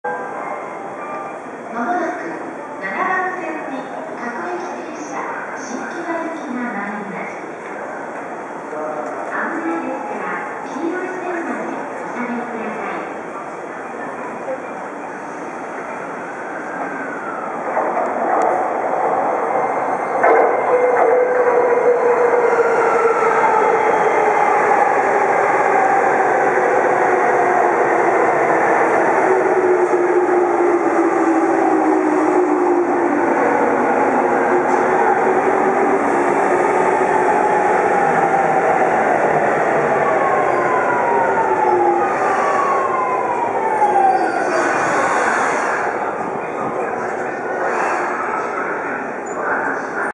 赤羽駅 りんかい線[東京臨海高速鉄道]70-000形 快速 新木場行き 到着
りんかい線